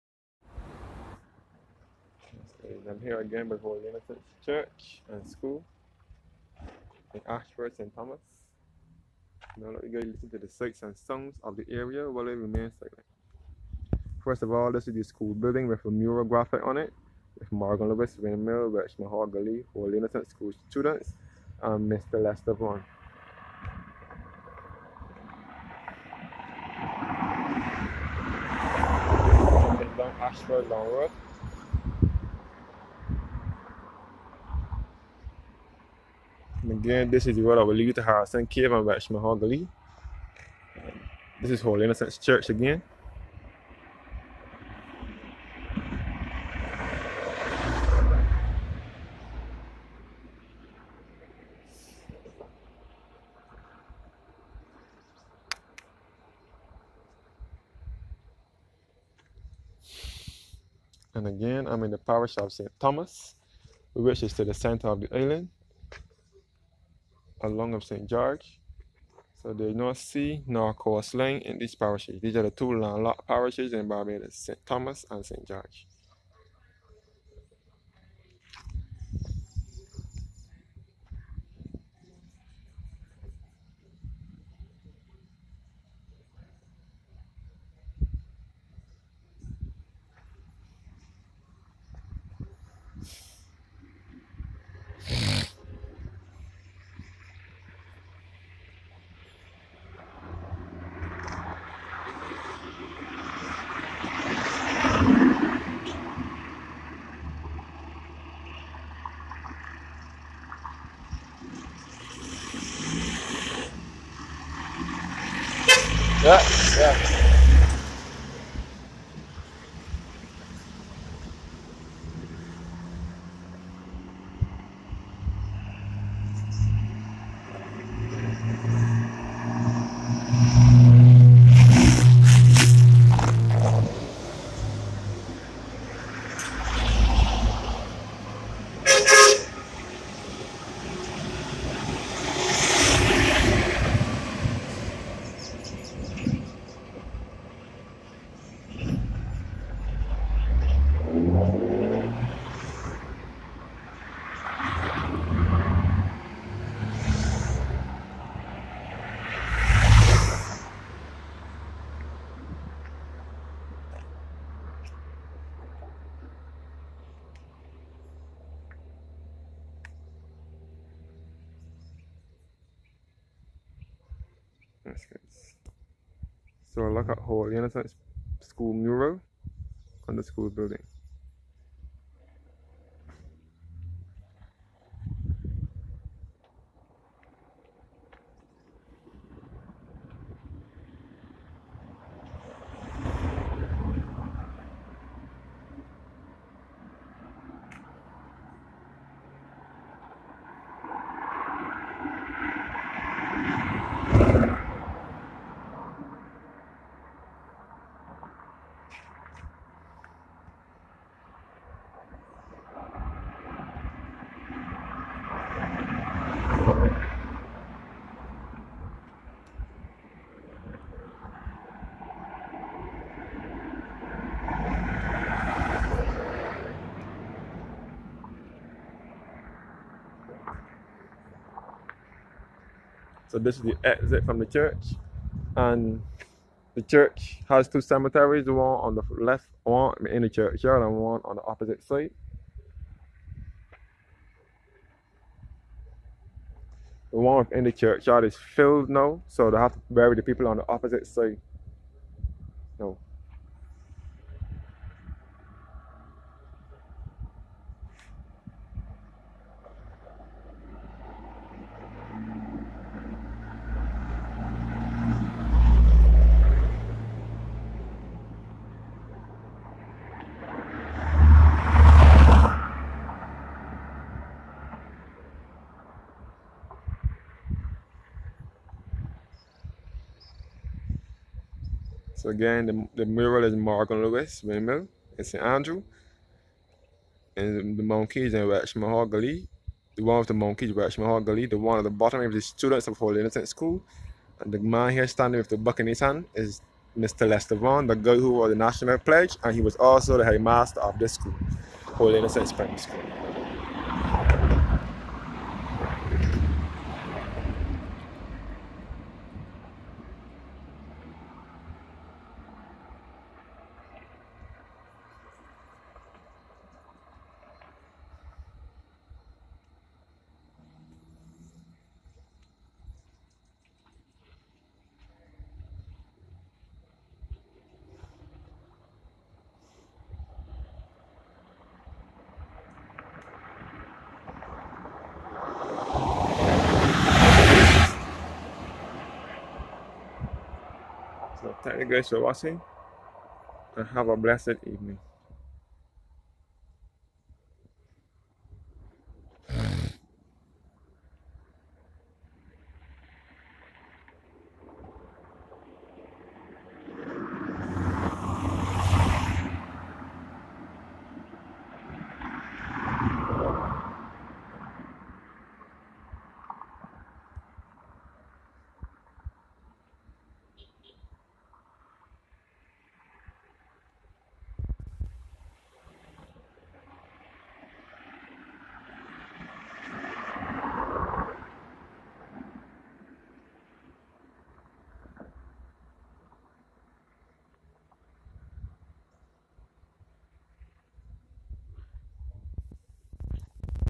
Holy Innocents Church & School Area Along Ayshford St. Thomas Barbados 🇧🇧 4
Holy Innocents is situated Along Ayshford St. Thomas on Highway2 on the way to Harrison's Cave and Welchman Hall Gully